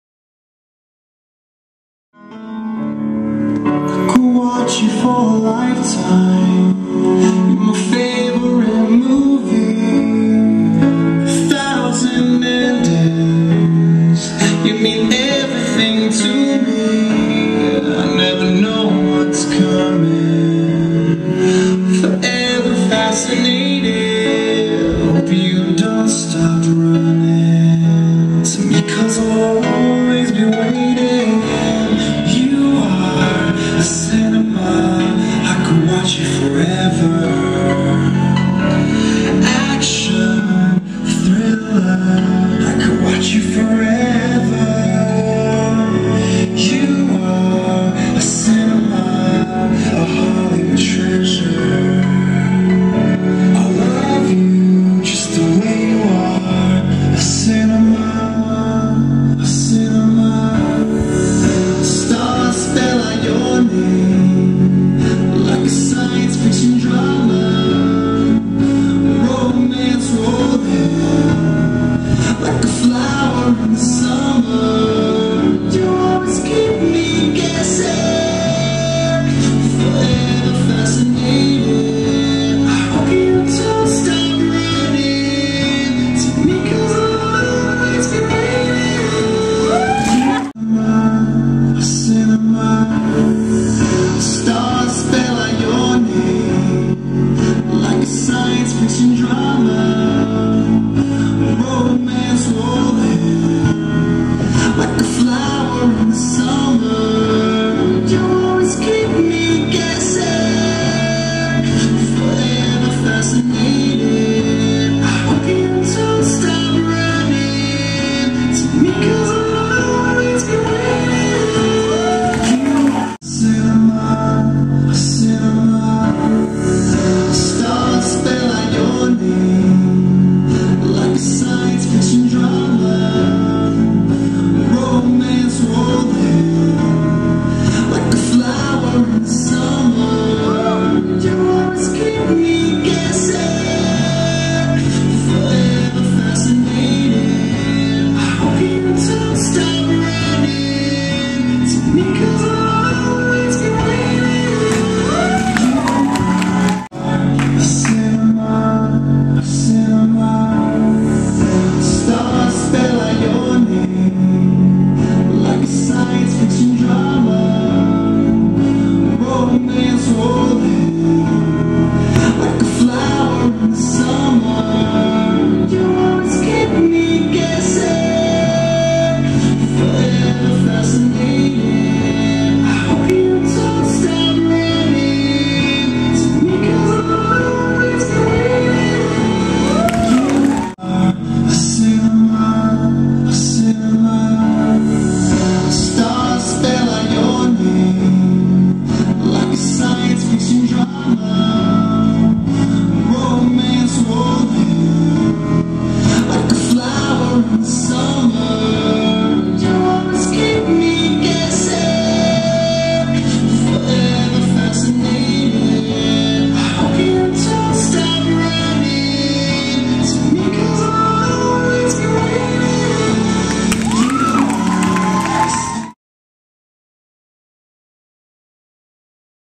forever fascinated
This class was a dedication and a celebration of the wedding of Maci Cameron to Cory Proctor. Matt and I choreographed this combo for them.